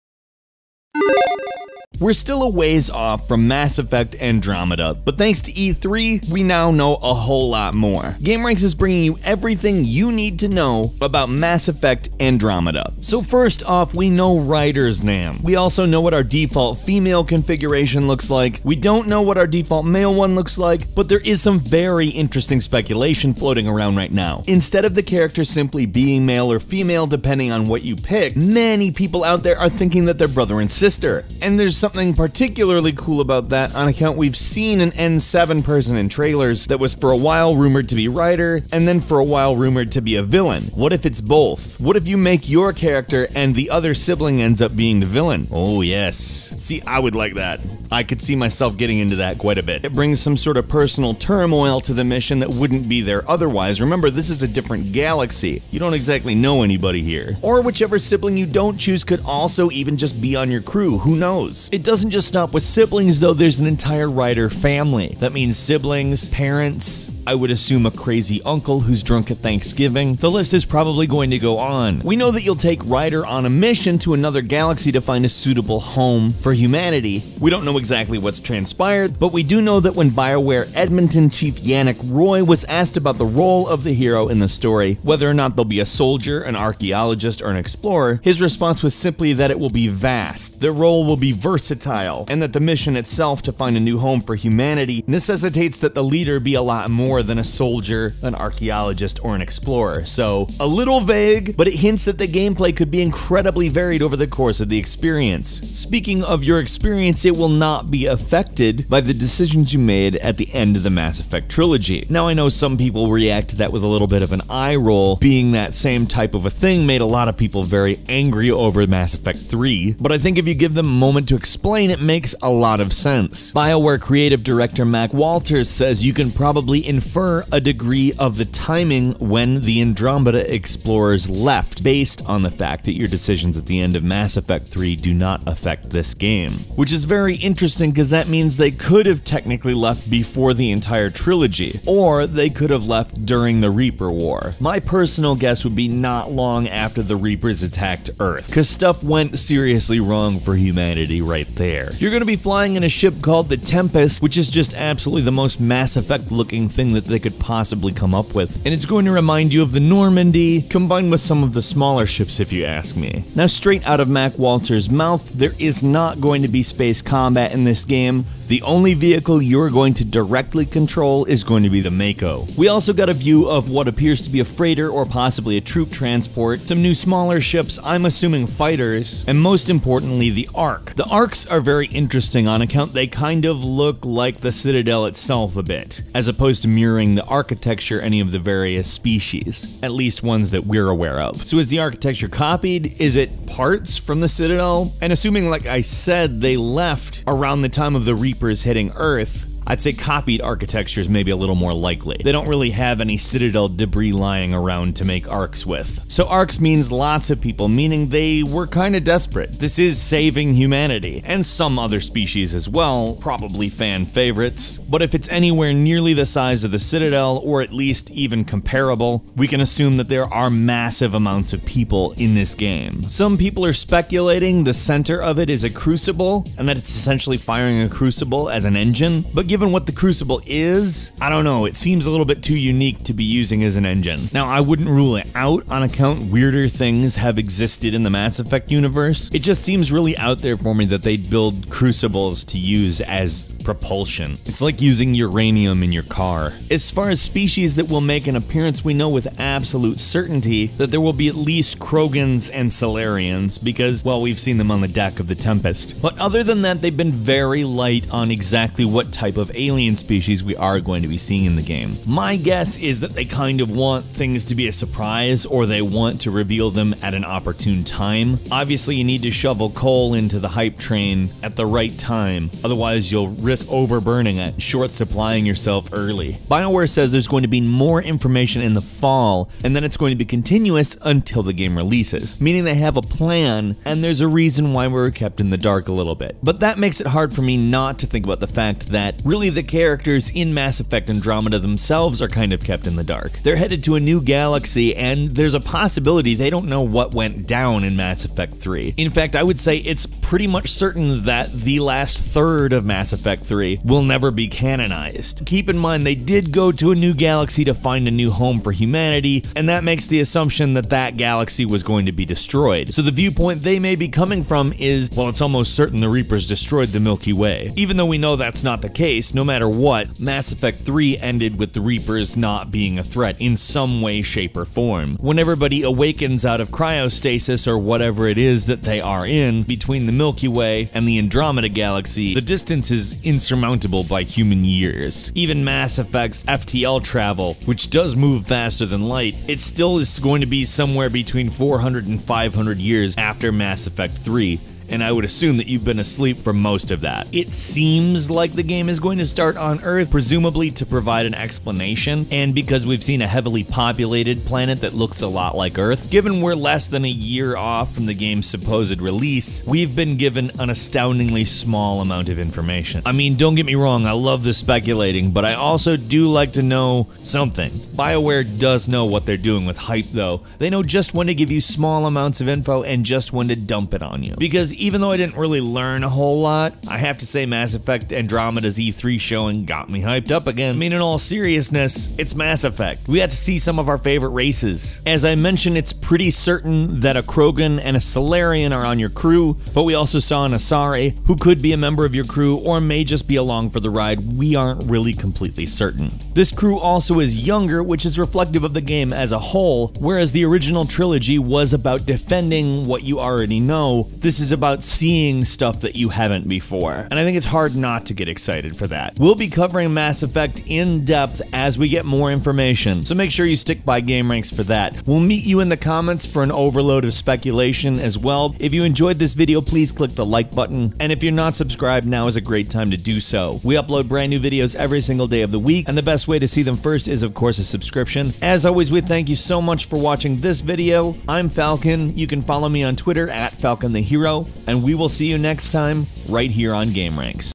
Mass Effect Andromeda: NEW Things We Learned
Mass Effect Andromeda had another vague showing at E3 2016, but we did manage to learn quite a few things. Falcon is here to break it down.